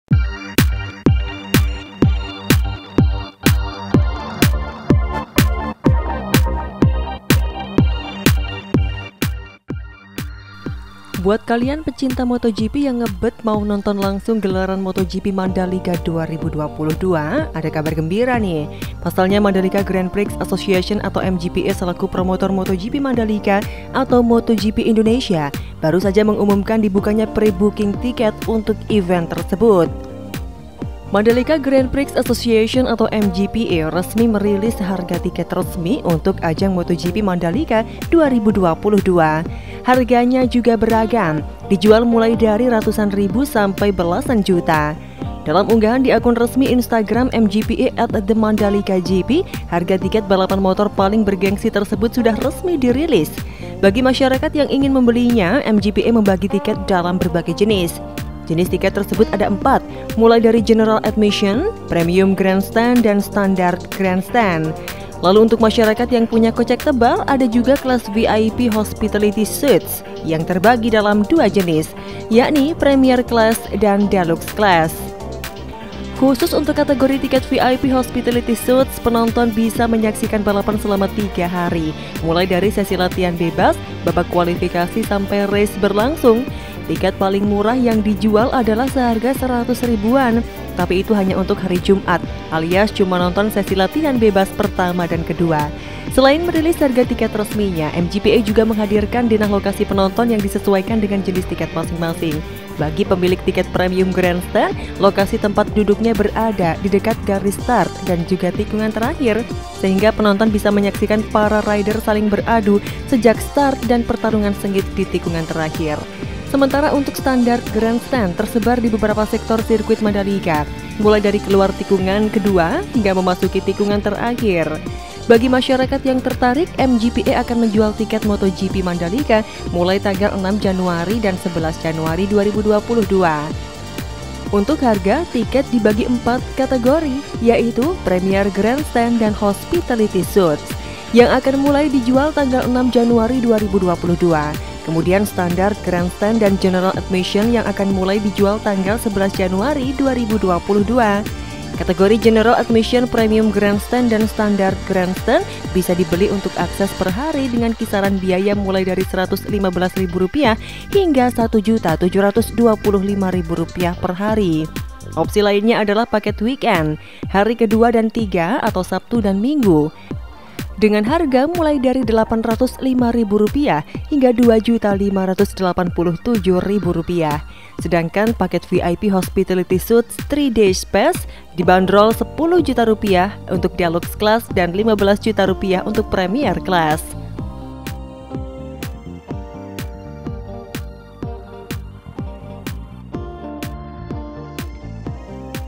LIHAT VIDEO INI❗SEBELUM BELI TIKET MotoGP SIRKUIT MANDALIKA
Buat kalian pecinta MotoGP yang ngebet mau nonton langsung gelaran MotoGP Mandalika 2021, ada kabar gembira nih.
Pasalnya, Mandalika Grand Prix Association (MGPA) selaku promotor MotoGP Mandalika atau MotoGP Indonesia baru saja mengumumkan dibukanya pre-booking tiket untuk event tersebut.

Mandalika Grand Prix Association (MGPA) resmi merilis harga tiket resmi untuk ajang MotoGP Mandalika 2022. Harganya juga beragam, dijual mulai dari ratusan ribu sampai belasan juta.
Dalam unggahan di akun resmi Instagram MGPA @themandalikagp, harga tiket balapan motor paling bergengsi tersebut sudah resmi dirilis. Bagi masyarakat yang ingin membelinya, MGPA membagi tiket dalam berbagai jenis.
Jenis tiket tersebut ada empat, mulai dari General Admission, Premium Grandstand, dan Standard Grandstand. Lalu untuk masyarakat yang punya kocek tebal, ada juga kelas VIP Hospitality Suites yang terbagi dalam dua jenis, yakni Premier Class dan Deluxe Class.

- Every Content in this Channel for purpose such as Education, News Report, interpretatio